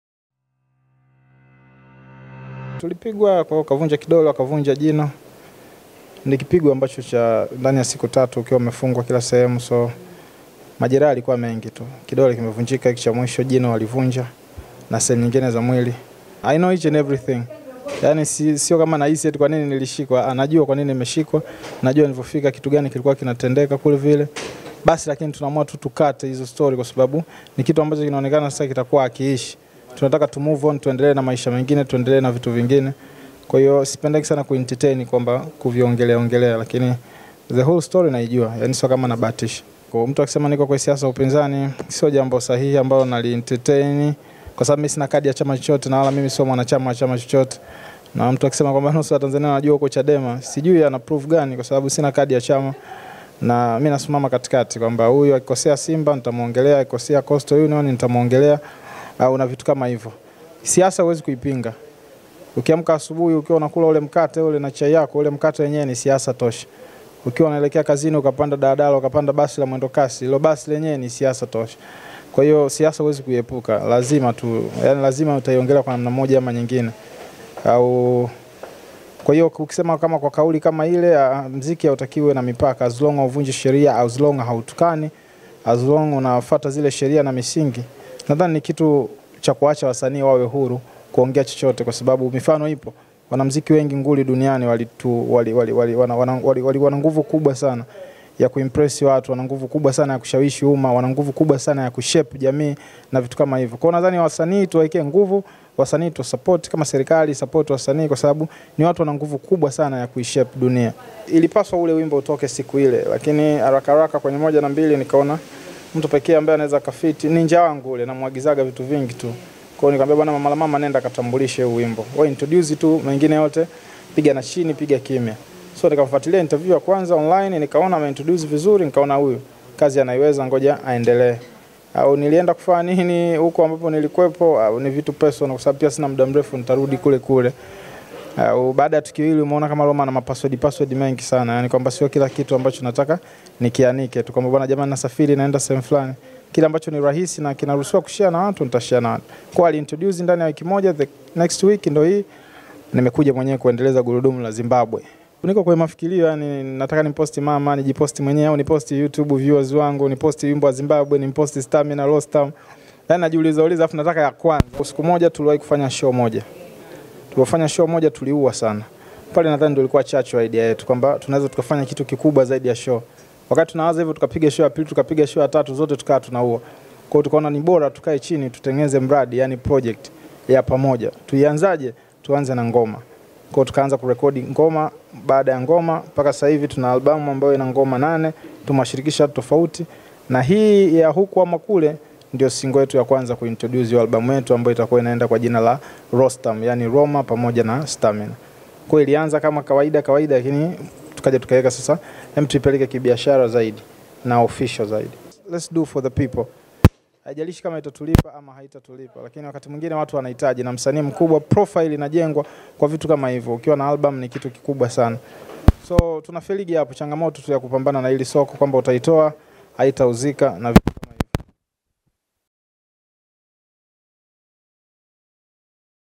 Interview : Roma afunguka ukweli
Roma aweka wazi kila kitu alichofanywa alipotekwa